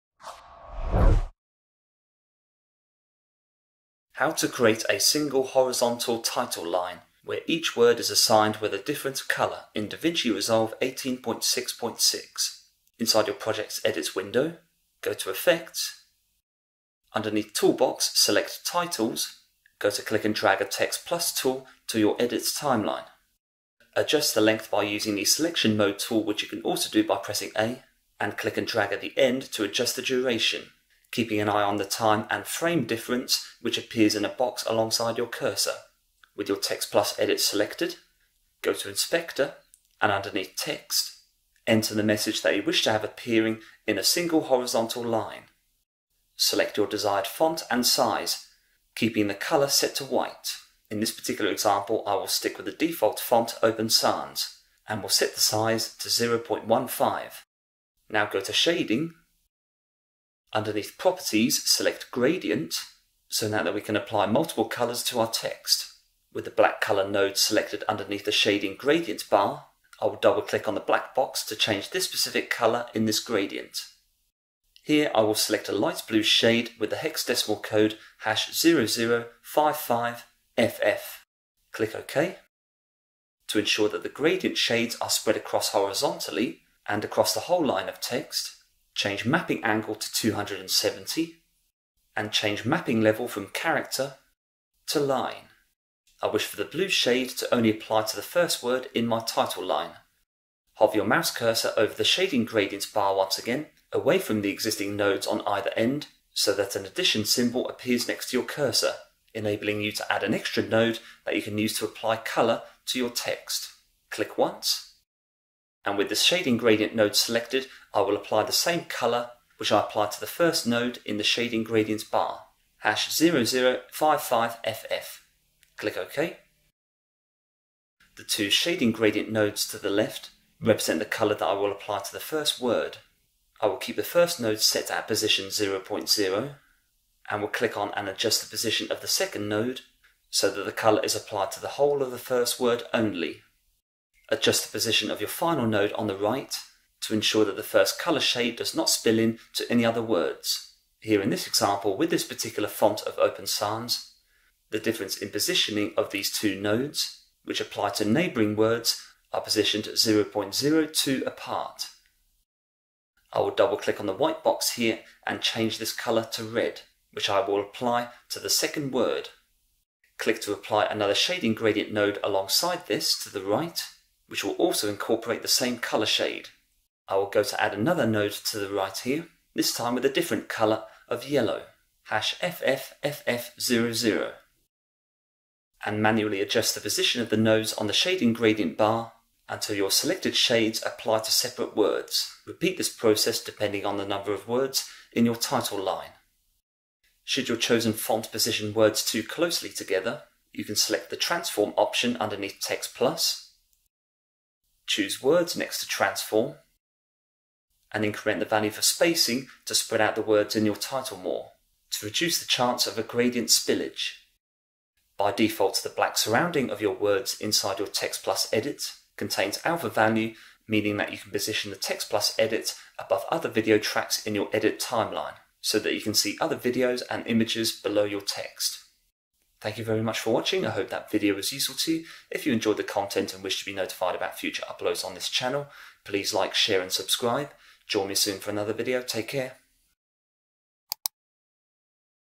DaVinci Resolve Tutorial: How To Create Titles With Different Coloured Words
DaVinci Resolve Tutorial #226. In this video, you will learn how to apply a gradient filter to short title text lines on an individual horizontal line. Version 18.6.6 of the software is used in this video.

***SPECIAL OFFERS

3) VISUALVFX

C0NTI